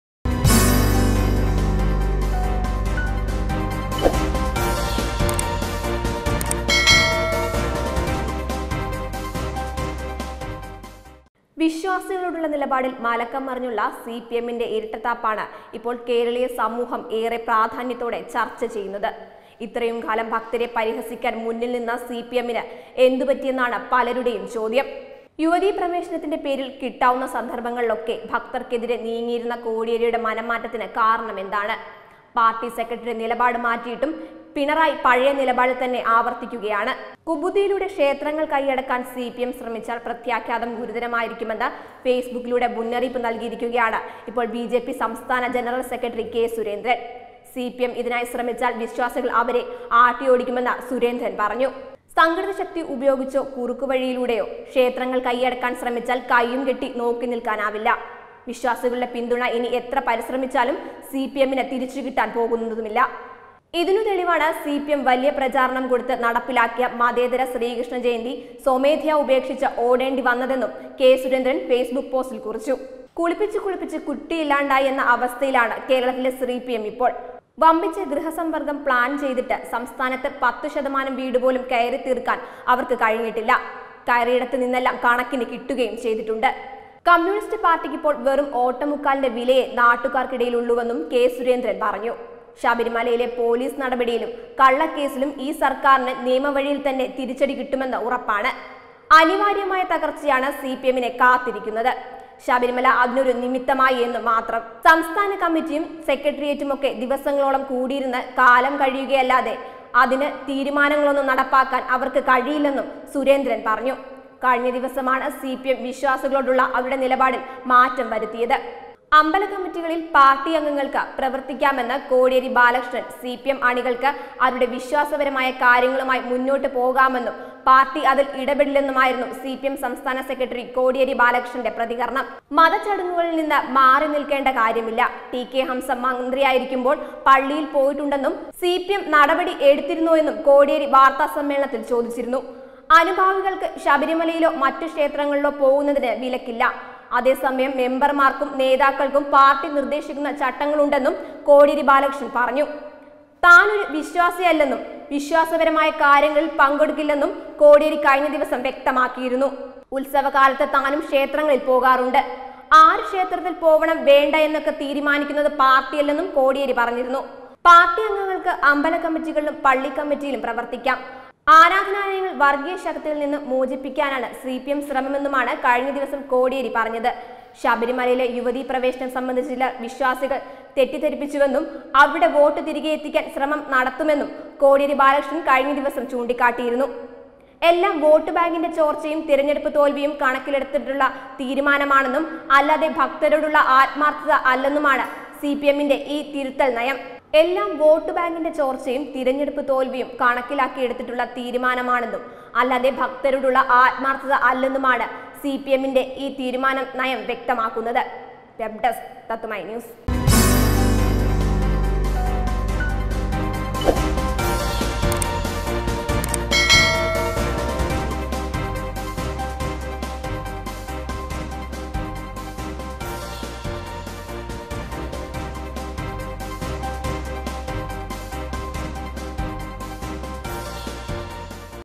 ക്ഷേത്രങ്ങള്‍ വിശ്വാസികളുടേത്; ഗുണ്ടായിസം കാണിച്ചാല്‍ നോക്കി നില്‍ക്കില്ല
കുബുദ്ധിയിലൂടെ ക്ഷേത്രങ്ങള്‍ കൈയടക്കാന്‍ സിപിഎം ശ്രമിച്ചാല്‍ പ്രത്യാഘാതം ഗുരുതരമായിരിക്കുമെന്ന് ഫേസ്ബുക്കിലൂടെ മുന്നറിയിപ്പ് നല്‍കിയിരിക്കുകയാണ് ഇപ്പോള്‍ ബി ജെ പി സംസ്ഥാന ജനറല്‍ സെക്രട്ടറി കെ സുരേന്ദ്രന്‍. സി പി എം ഇതിനായി ശ്രമിച്ചാല്‍  വിശ്വാസികള്‍ അവരെ ആട്ടിയോടിക്കുമെന്ന് സുരേന്ദ്രന്‍ പറഞ്ഞു. സംഘടിതശക്തി ഉപയോഗിച്ചോ കുറുക്കുവഴിയിലൂടെയോ ക്ഷേത്രങ്ങള്‍ കയ്യടക്കാന്‍ ശ്രമിച്ചാല്‍ കയ്യും കെട്ടി നോക്കി നില്‍ക്കില്ല